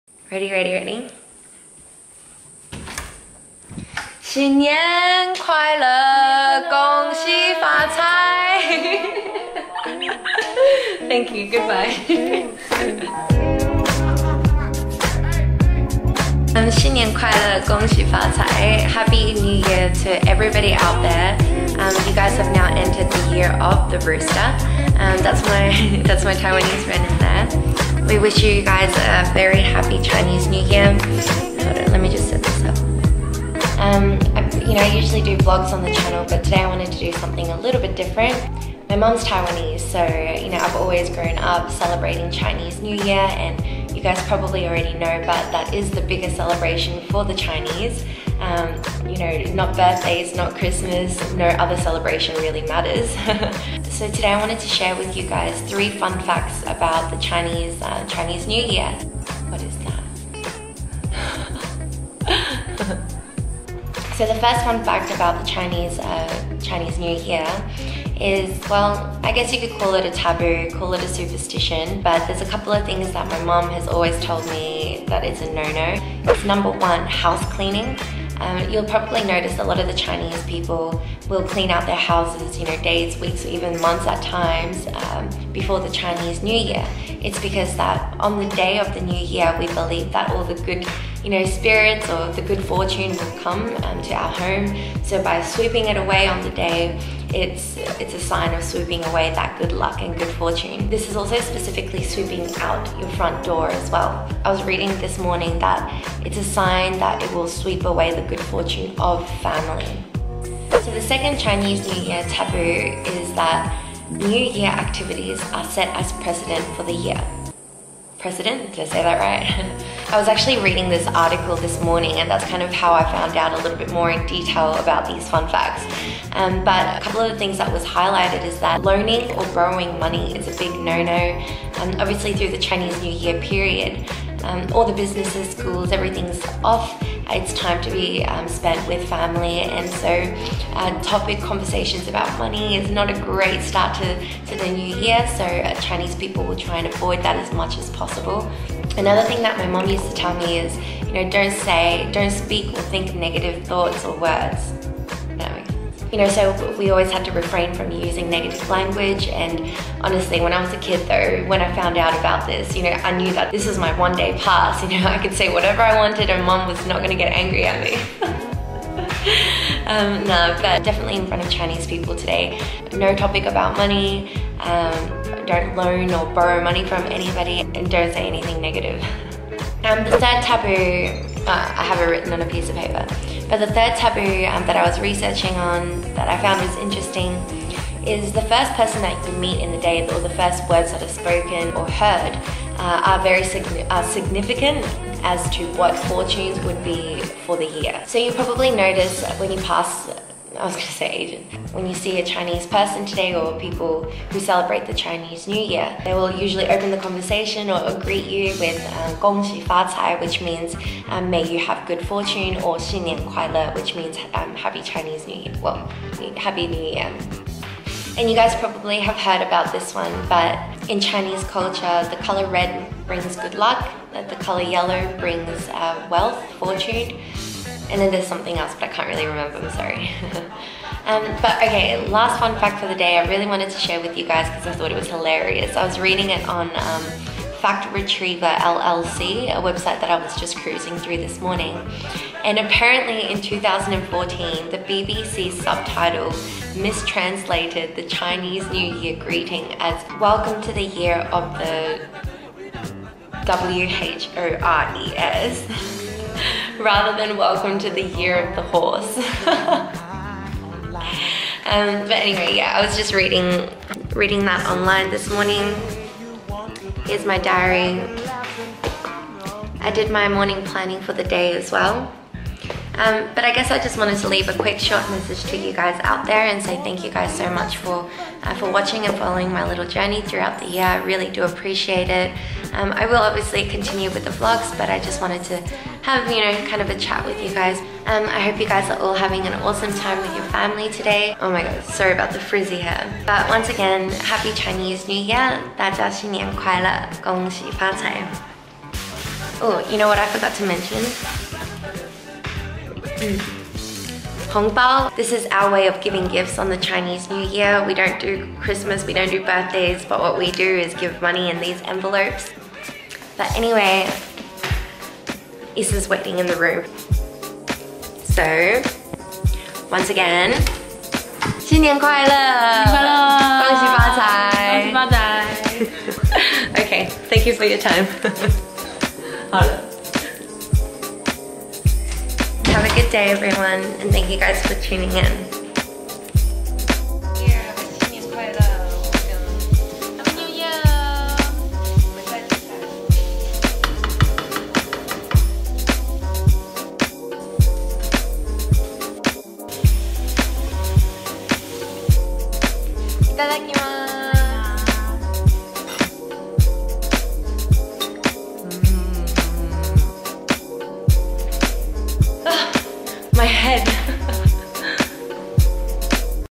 Mimi-T: 3 FACTS - CHINESE NEW YEAR | Superstitions & Taboos | 恭禧發財
I haven't recently posted any sit down, talking vlogs - but just wanted to share bits and pieces of my Chinese New Year experiences :) Thankyou for your support through the year.

2016 was a tough year as most could agree.. and now to the next chapter of our life we keep on moving forward. You've been the support behind the scenes.

Thanking you kindly,

Mimi